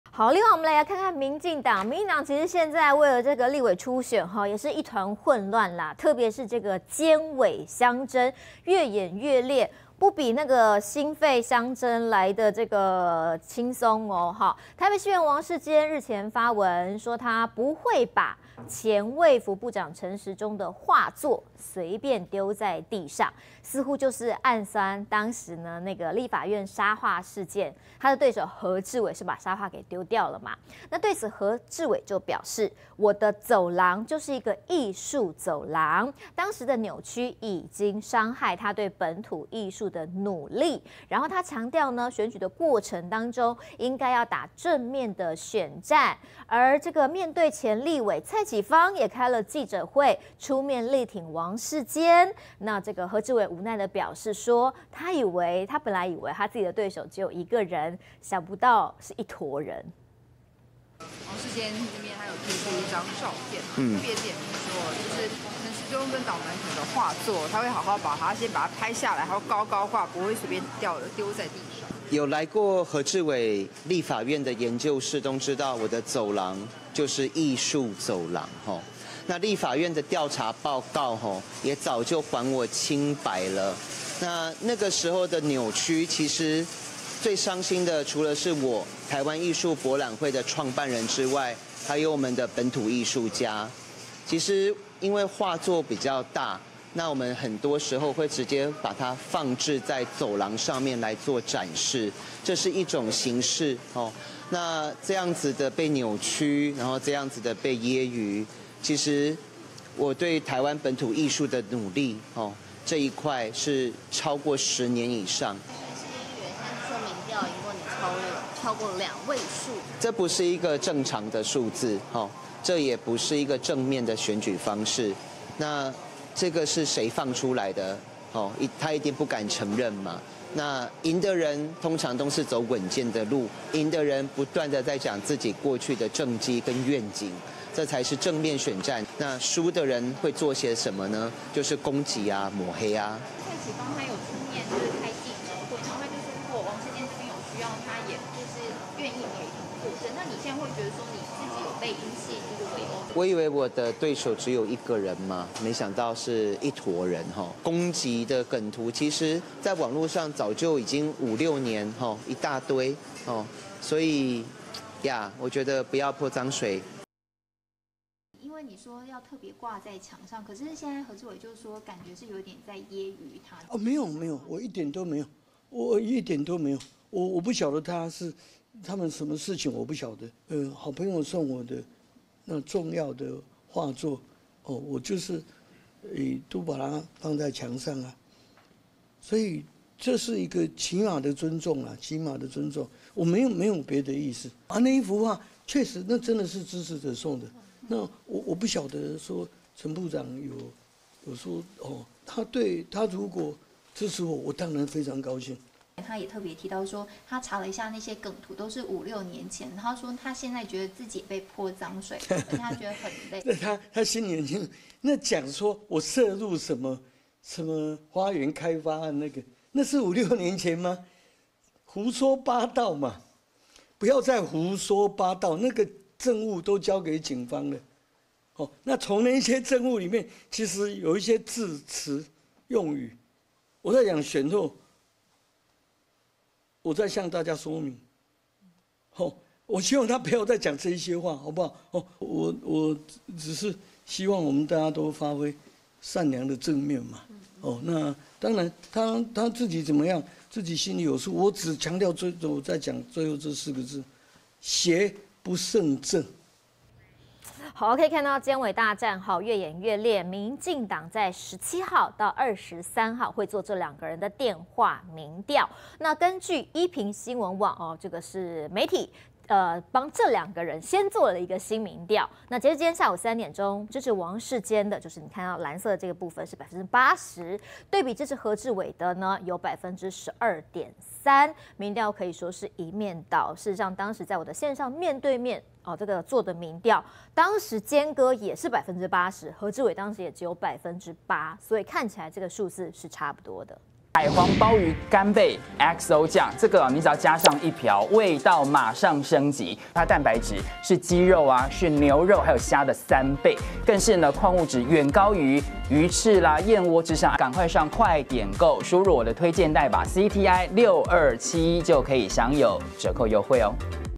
【每日必看】北市立委藍"坐7搶8" 主委坦言:王世堅最棘手｜民調一面倒? 支持度王世堅80% 何志偉12.3% 20230415
00:00 北市立委藍"坐7搶8" 主委坦言:王世堅最棘手
02:50 暗酸何志偉"把畫丟地上"? 王世堅否認:沒這意思
04:18 民調一面倒? 支持度王世堅80% 何志偉12.3%